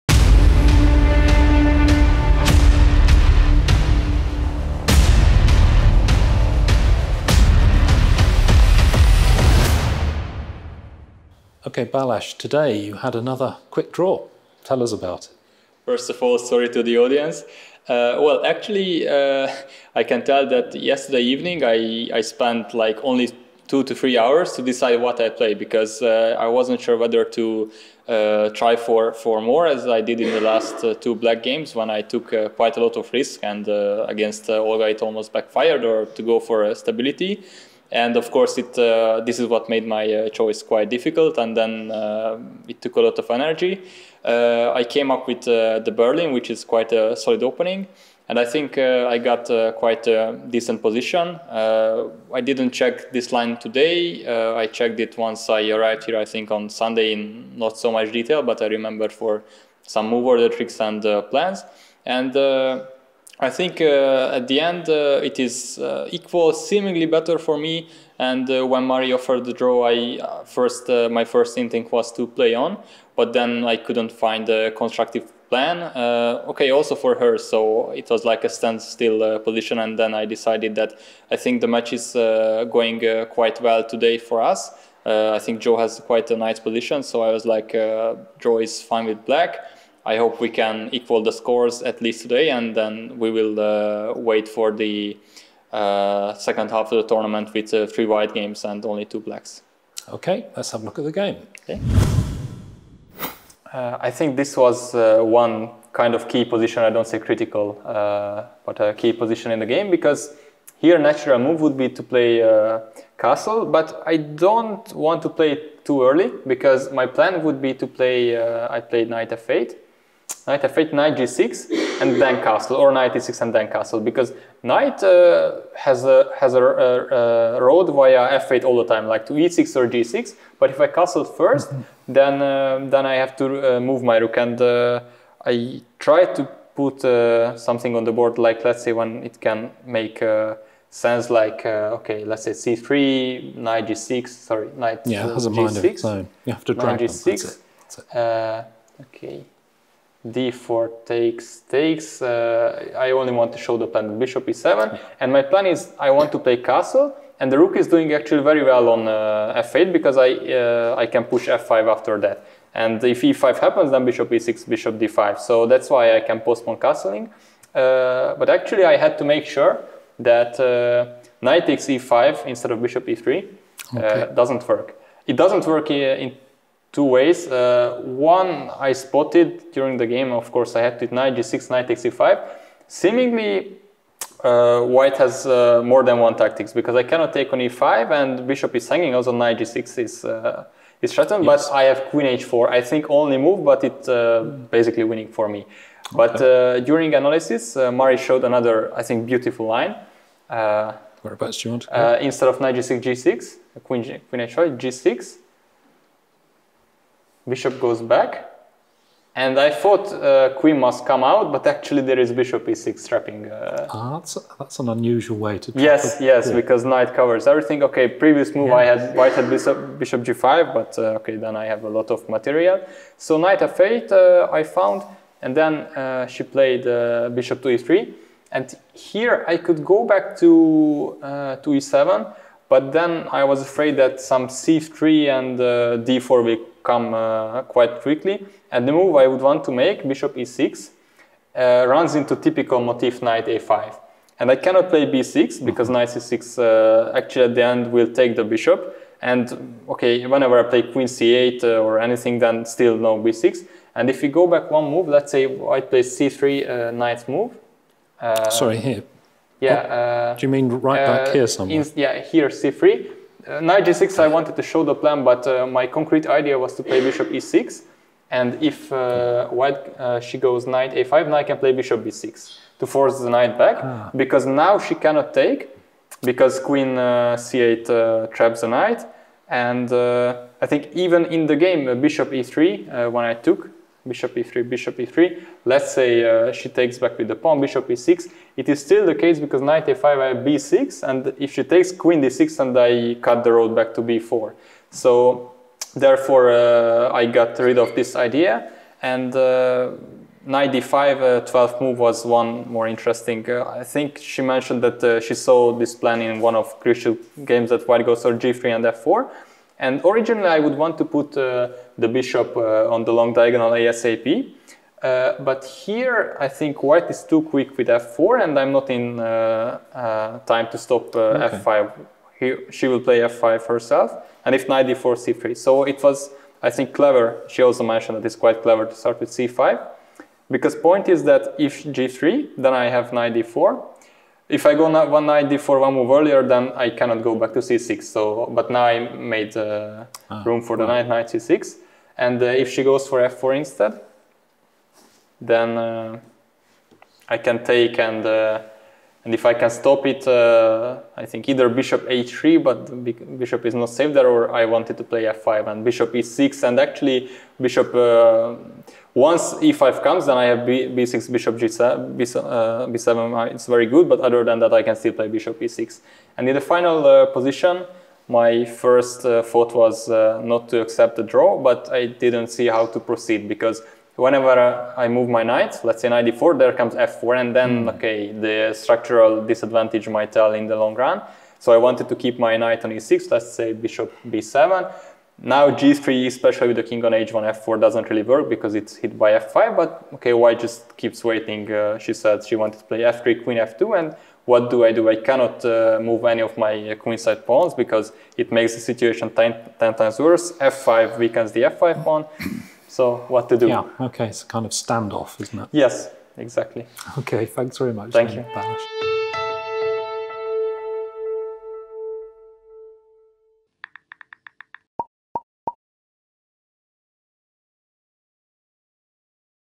GibChess Battle of the Sexes, Round 5, interview with Balazs Csonka
Interview with Hungarian IM Balazs Csonka at the GibChess Battle of the Sexes, Round 5, 28 January 2022 at the Garrison Library, Gibraltar. Interviewer John Saunders.